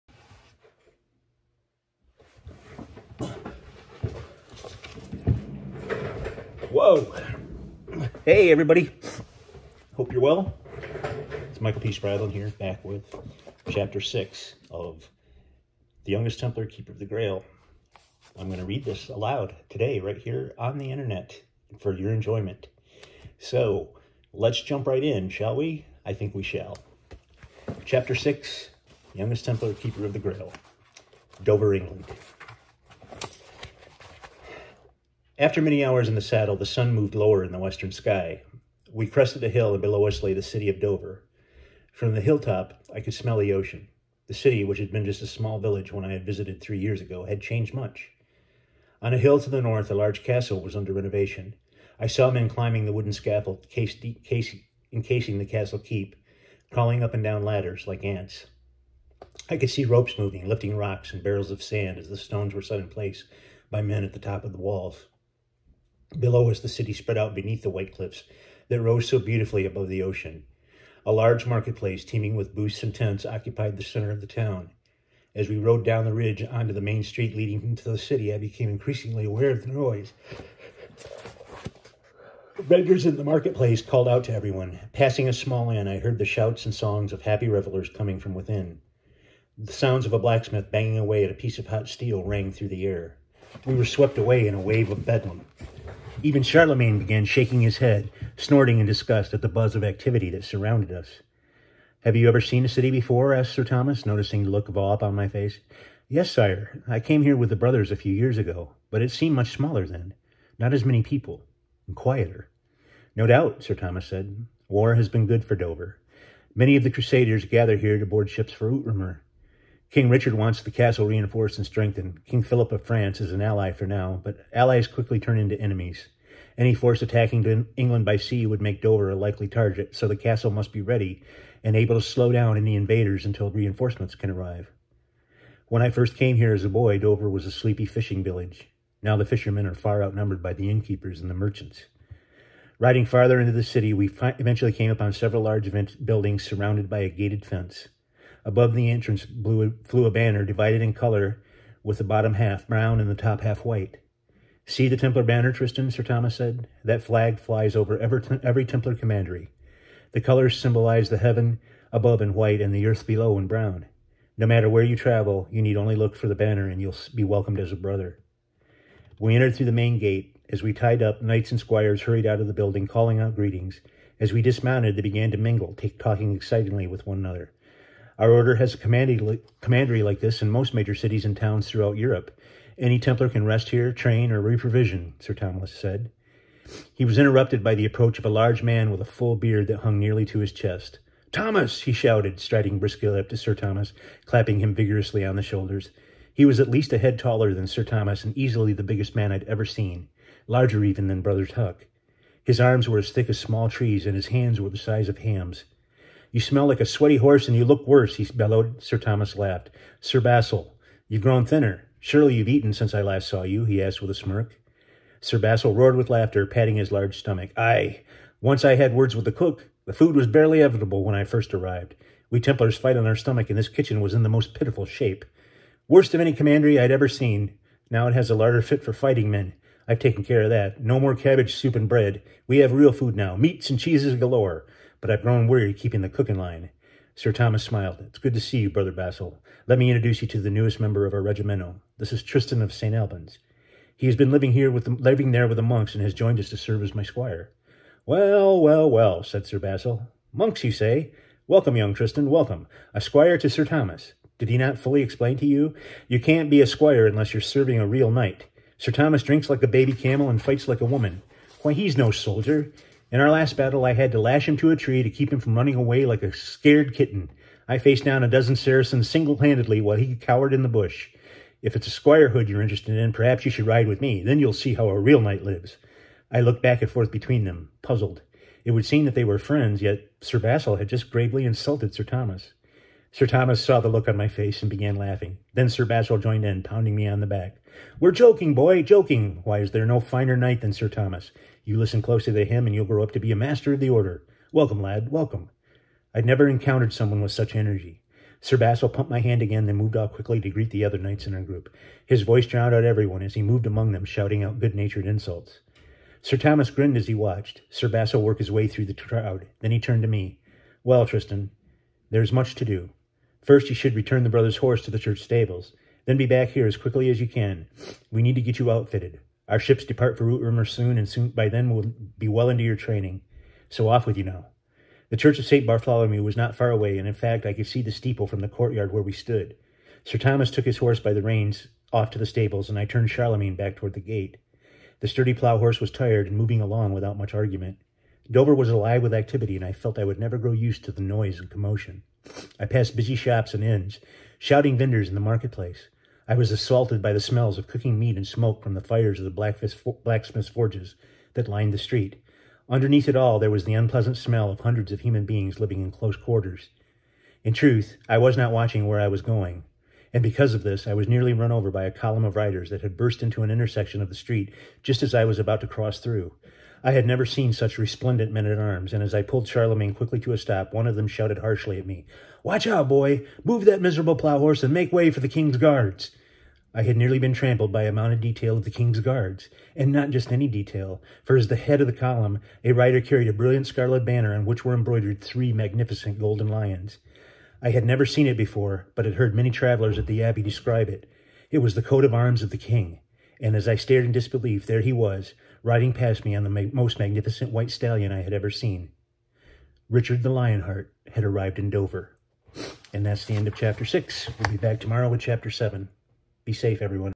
THE YOUNGEST TEMPLAR: Keeper of the Grail--Chapter Six
Michael P. Spradlin reads from his novel The Youngest Templar: Keeper of the Grail.